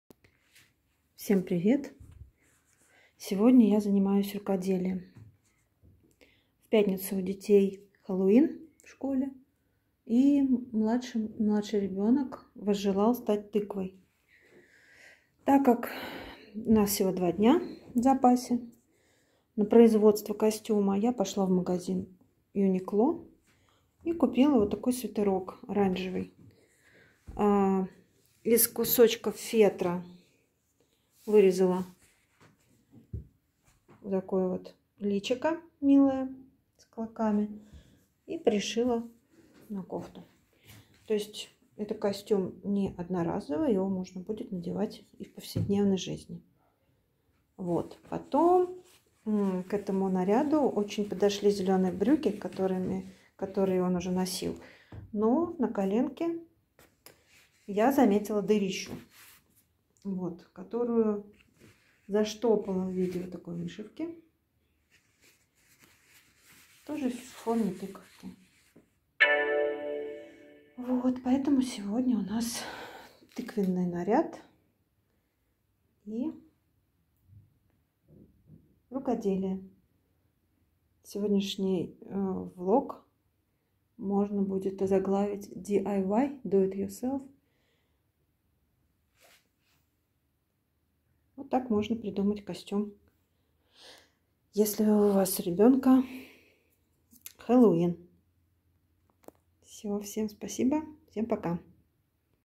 DIY костюм тыквы на Helloween
Это видео для тех, у кого нет времени на поиски костюма, а также для тех, кто не готов покупать одноразовый готовый наряд.. Быстро, просто, эффектно и практично...